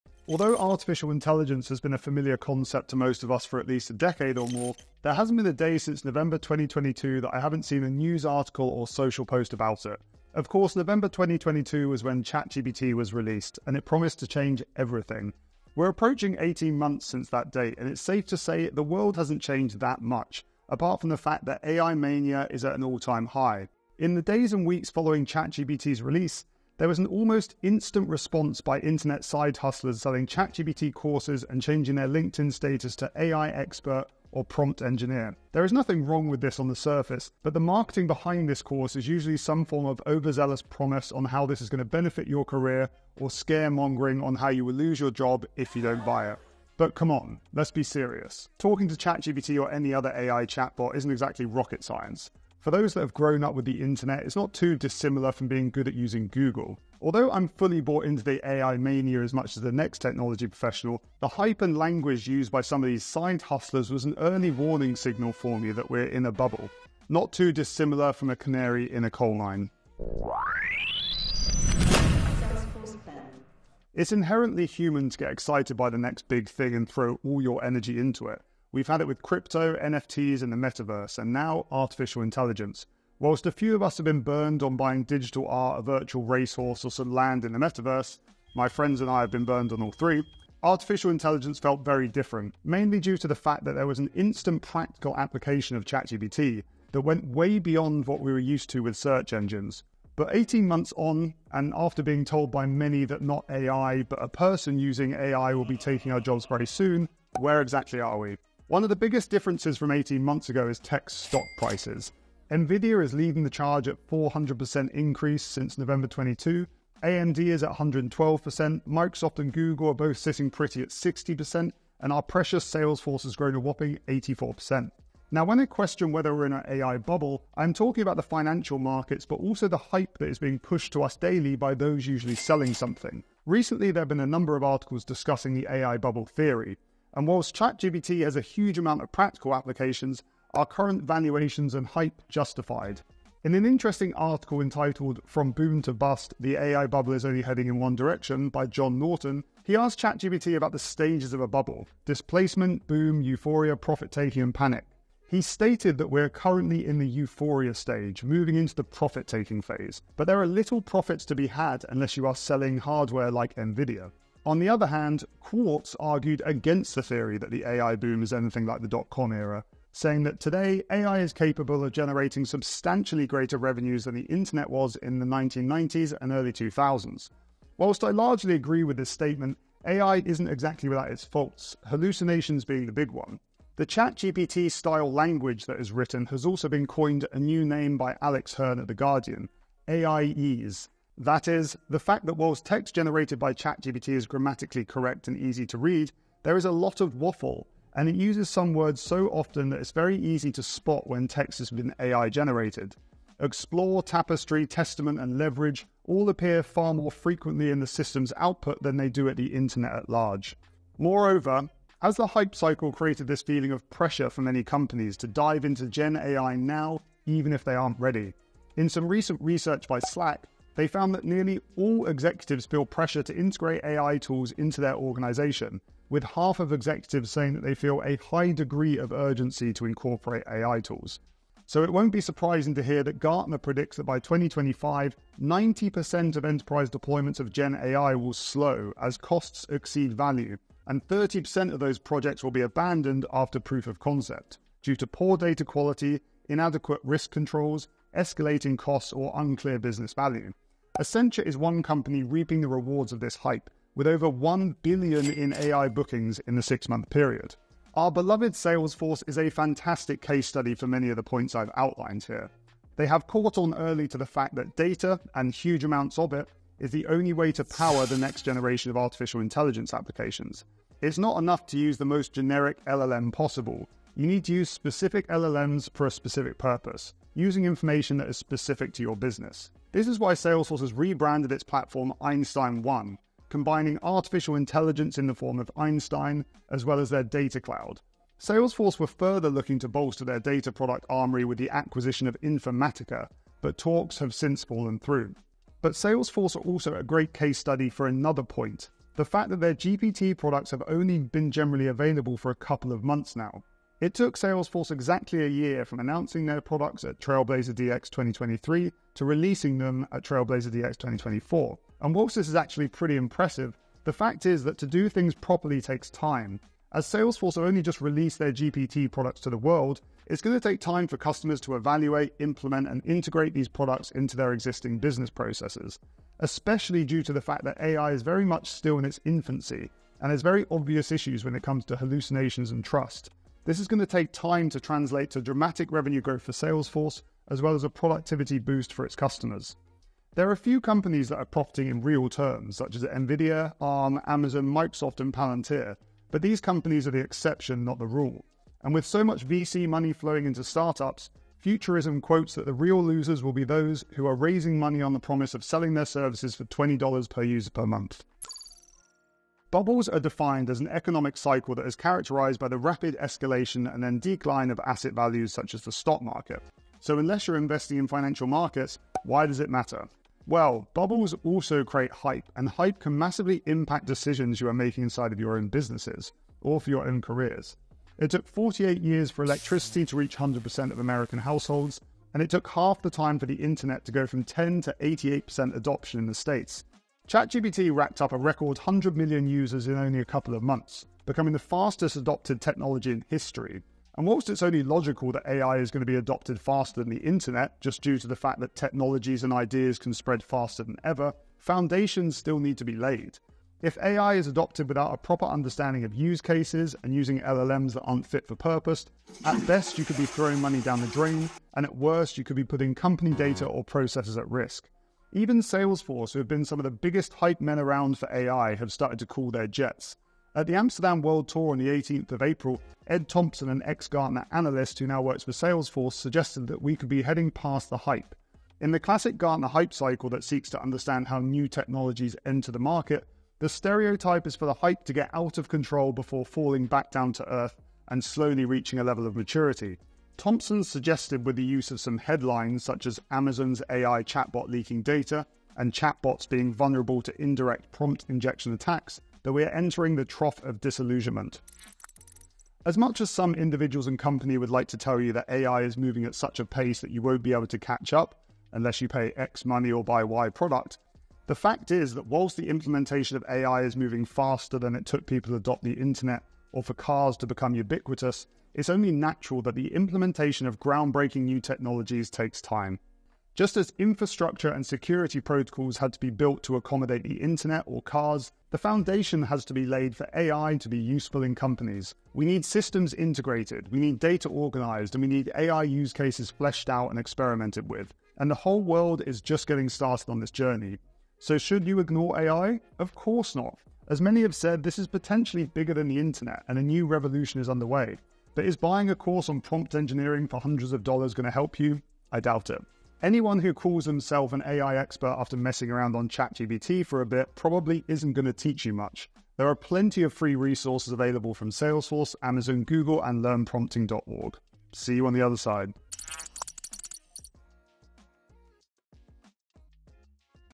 Are We in a Dot-Com Style Bubble for Artificial Intelligence?
No one can deny that since last year, the world has been fascinated by artificial intelligence – and we're not just talking about the Salesforce ecosystem either.

Although we're fully bought into AI-mania, the hype and language used by some of these side hustlers was an early warning signal that we are in a bubble. Here's why that's important.

Prefer reading?